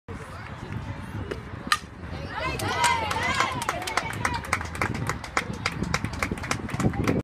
Kodi Forish hit to opposite field
Buckeye Recruitfest July 2015
All Star Game